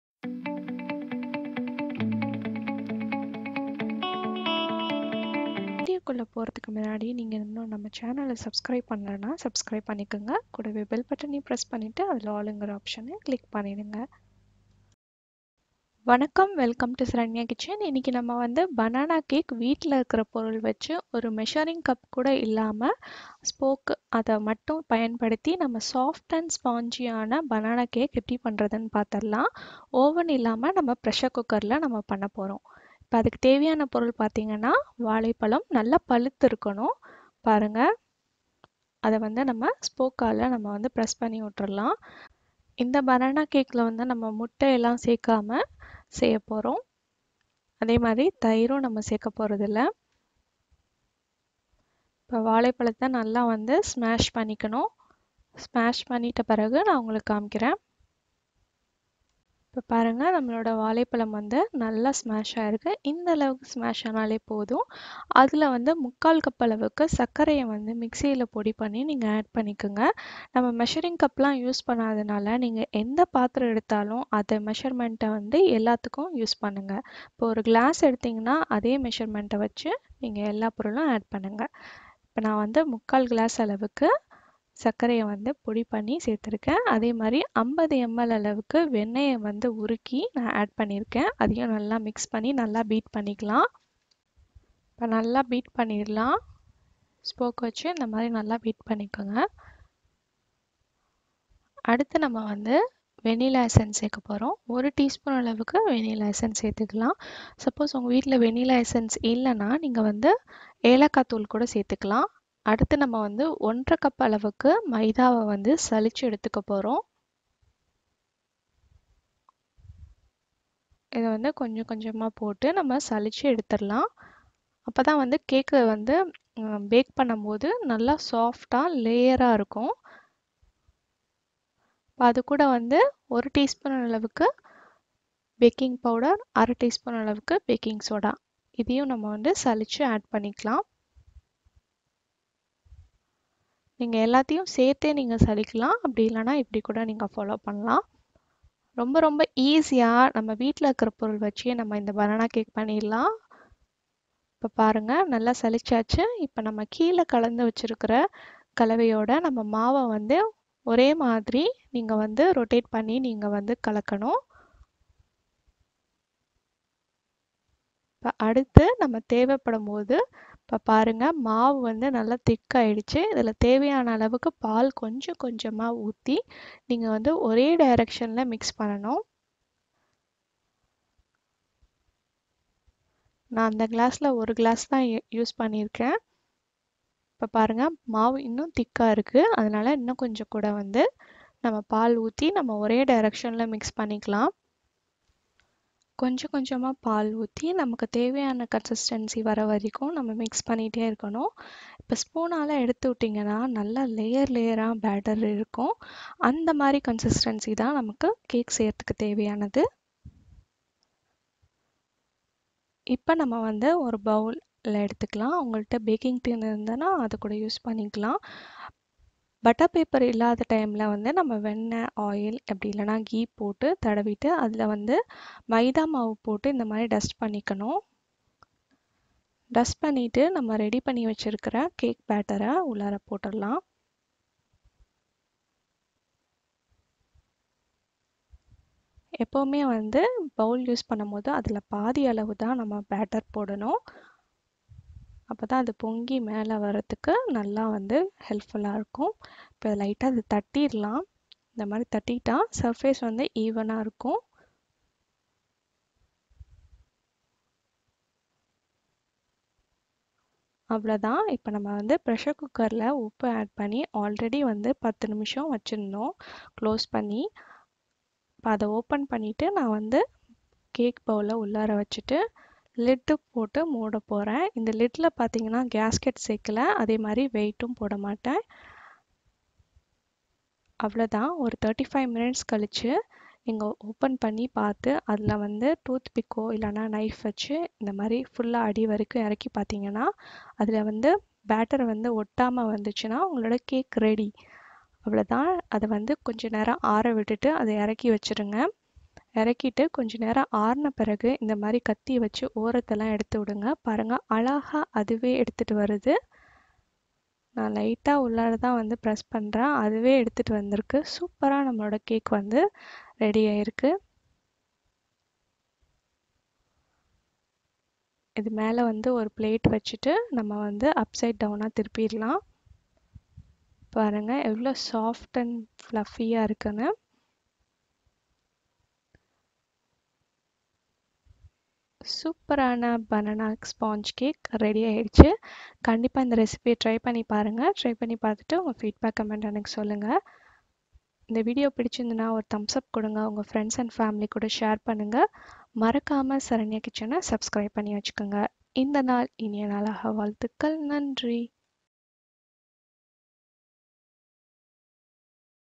Eggless Banana Cake in pressure cooker without measuring cup |NO OVEN|no egg cake recipe
Eggless banana cake is made without measuring and egg do try and share your feedback.
Keep Supporting
With Love
Saranya